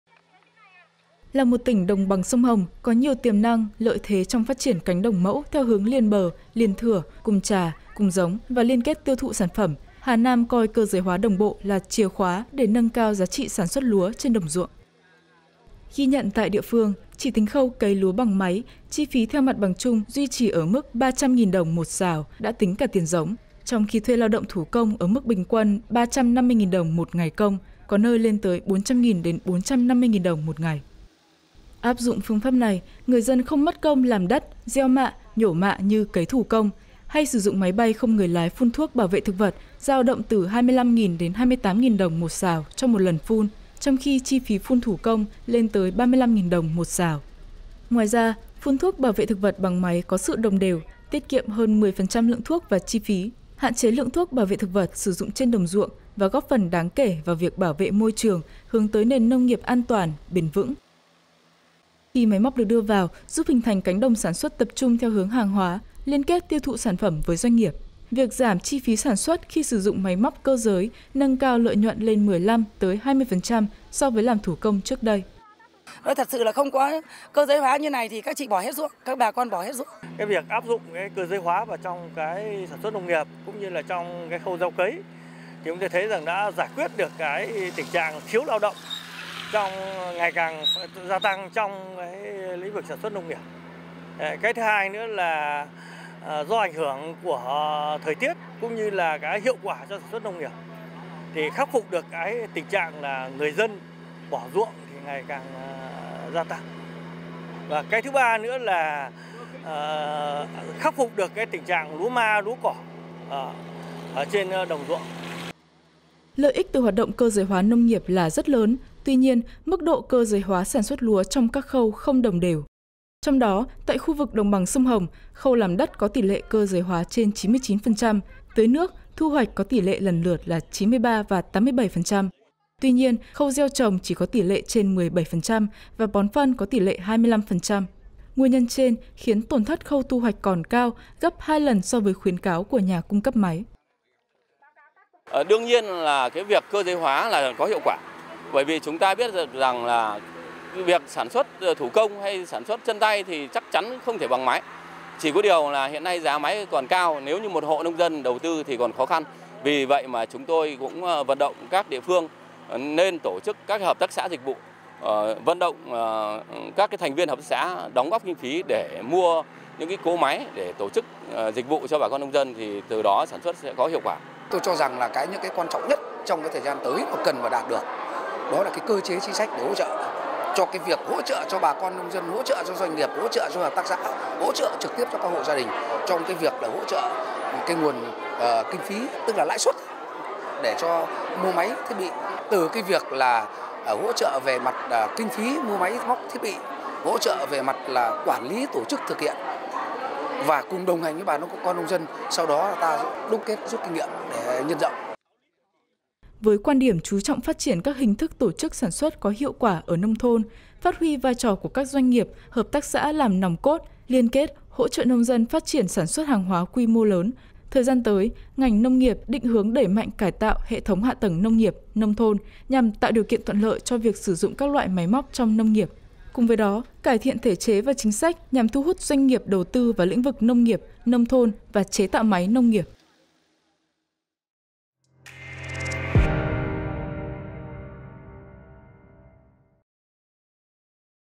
Tỷ lệ cơ giới hóa khâu gieo trồng lúa tại ĐBSH mới đạt 17%
Cục Kinh tế Hợp tác và Phát triển nông thôn cho biết, khu vực Đồng bằng sông Hồng, khâu làm đất có tỷ lệ cơ giới hoá trên 99%, tưới nước, thu hoạch có tỷ lệ lần lượt là 93% và 87%. Tuy nhiên, khâu gieo trồng và bón phân chỉ có tỷ lệ trên 17% và 25%.